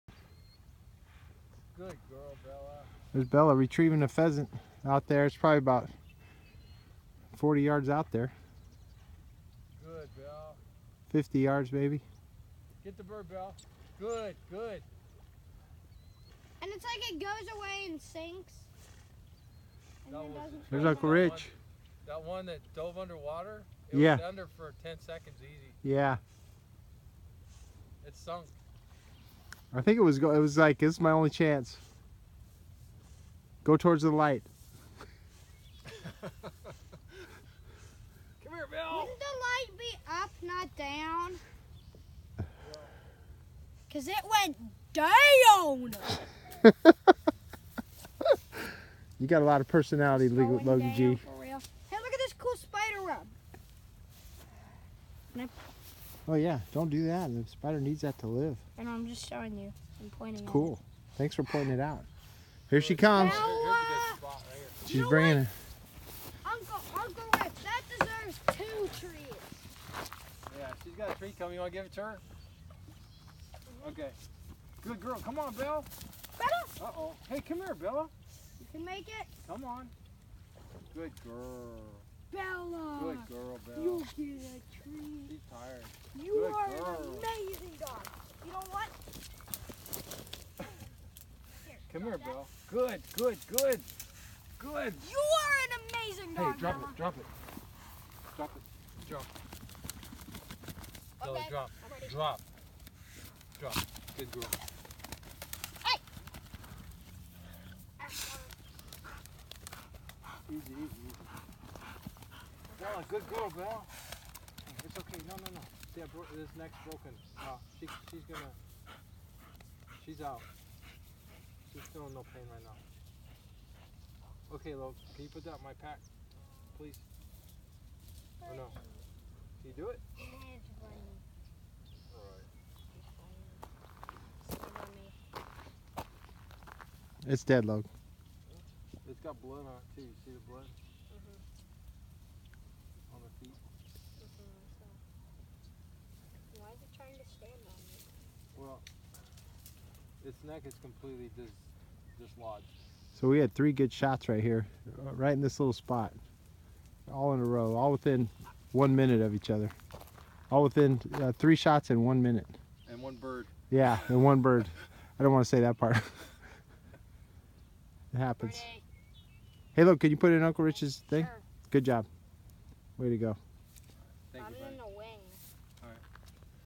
Hastings Island Pheasant water retrieve
Yellow lab Bella retrieves a pheasant